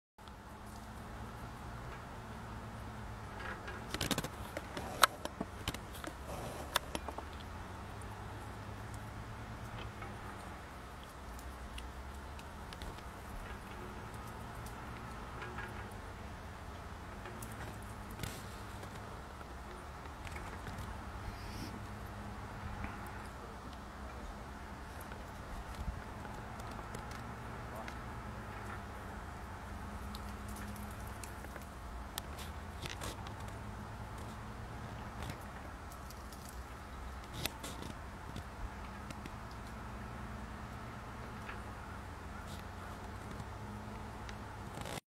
Here comes the snow plow!
Paul Montgomery captured this plow hard at work on Route 20 in Park Corner, P.E.I.